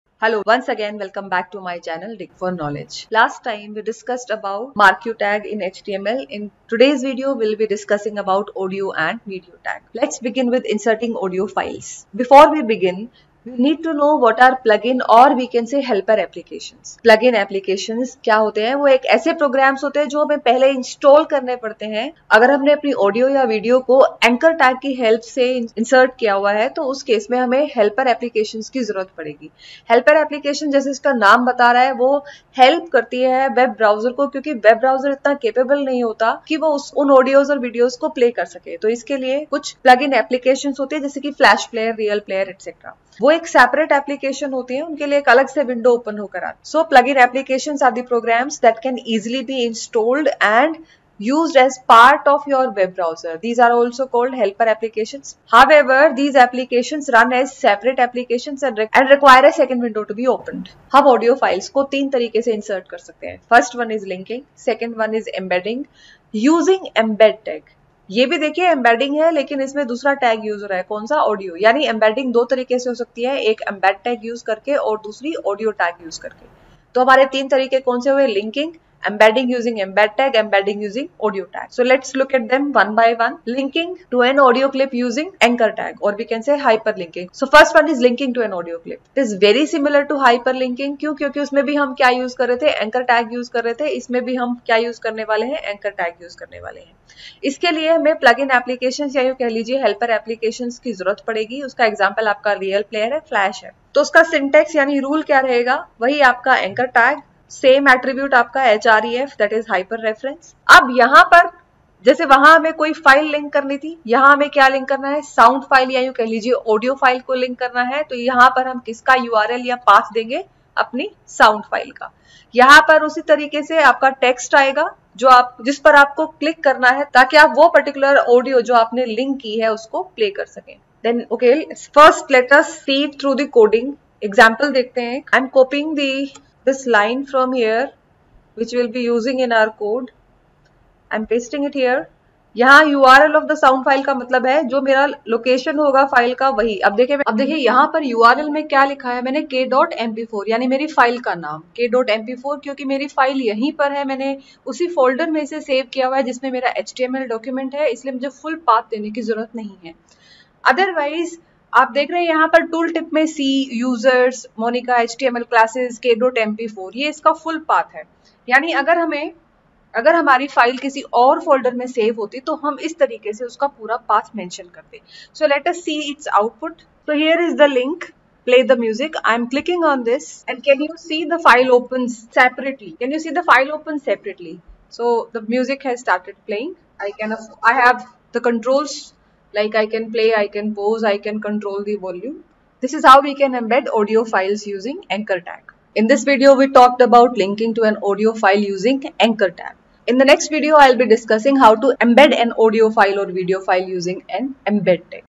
inserting audio and video in html| Inserting Audio in HTML using Anchor tag |how to add audio tag
dig4knowledge class10 dig insertingaudioandvideo insertingaudioinhtml audio and video in html| Inserting Audio in HTML using Anchor tag |how to add audio tag
audio tag - html 5 tutorial in hindi  | Complete Use in Hindi
we will learn to insert audio in our html file using video tag in html.
also we will learn related attributes. like :- controls, autoplay, loop, and source.
watch this video and learn it all.